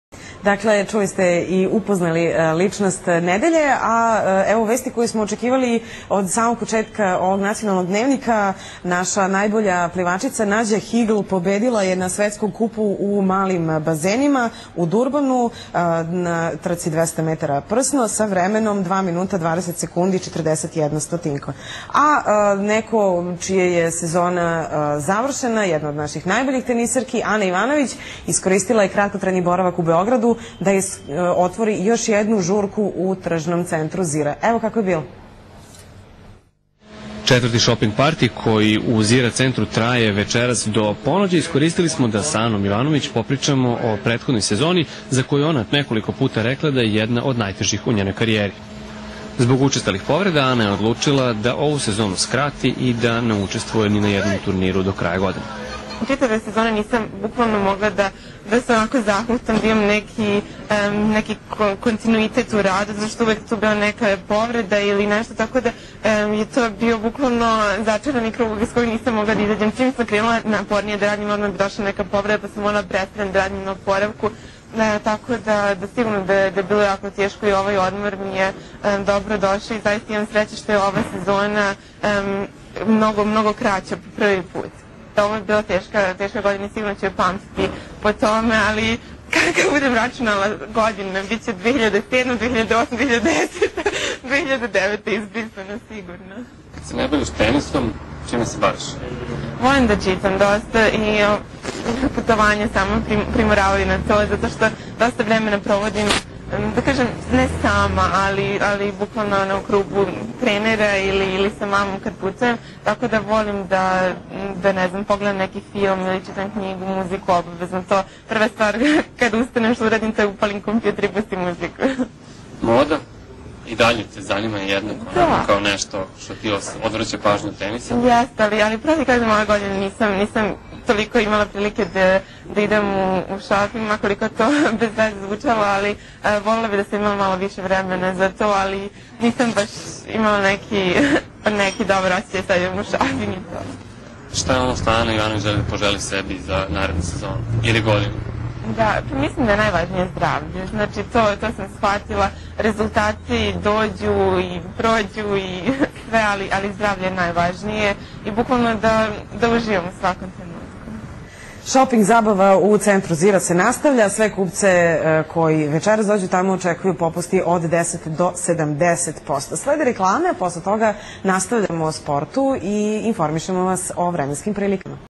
Ana Ivanovic interview on Pink TV
Ana Ivanovic gives interview to Pink TV while at Zira Center in Belgrade on October 17th, 2009.